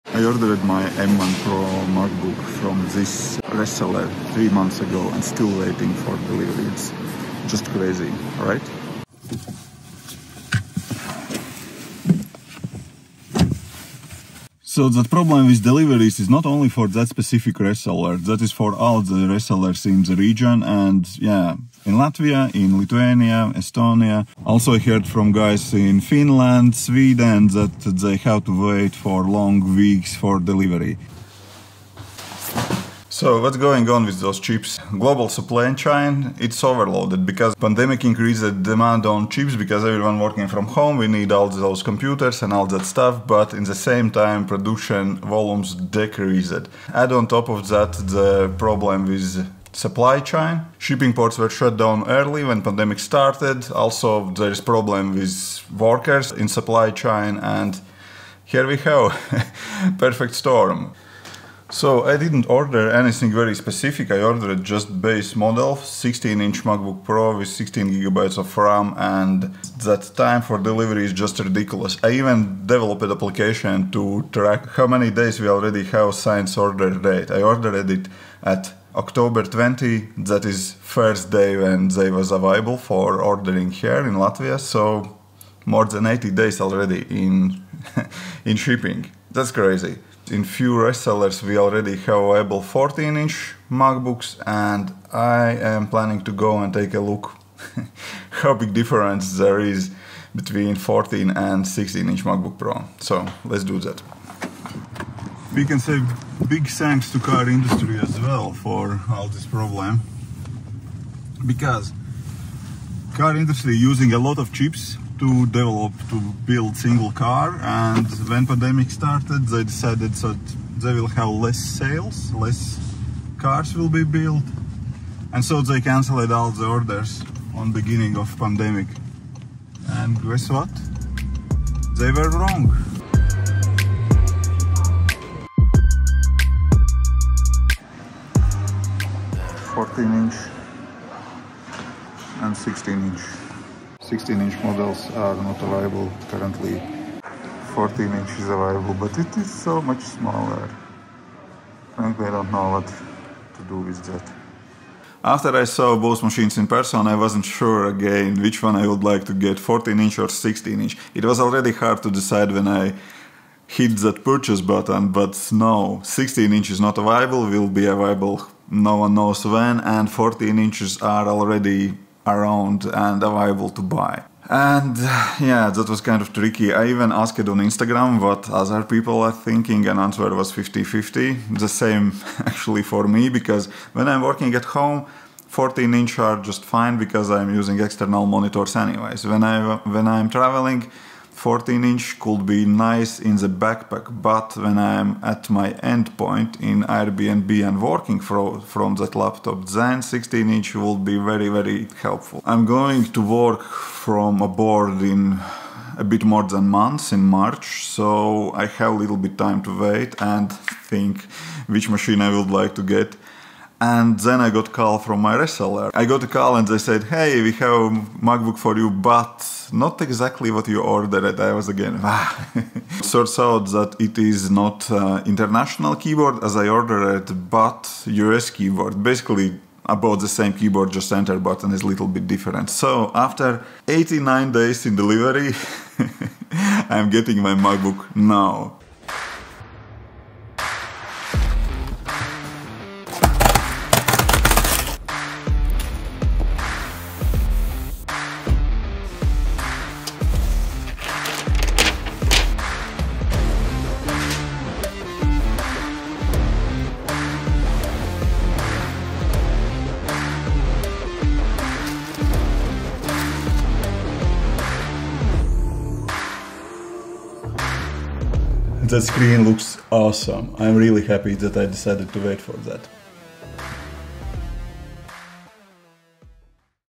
I was waiting 89 days to get this M1 Pro
I was on the fence between 14" and 16" M1 Pro MacBook till the last minute 😅

My Programming Setup:
CalDigit USB-C dock
💻 13” MBP M1 16GB Ram
Favorite Wide lens
HDMI to USB adapter for the camera
Favorite Microphone